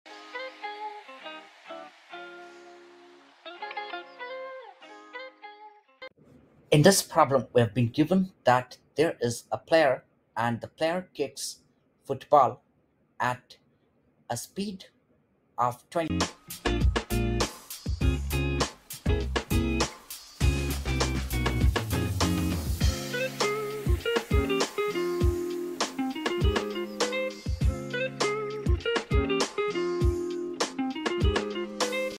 A player kicks a football from ground level with a velocity of magnitude 27.0 m/s at an angle of 30…
A player kicks a football from ground level with a velocity of magnitude 27.0 m/s at an angle of 30° above the horizontal. What is the horizontal distance the ball travels? (hint: d= 1/2 at^2)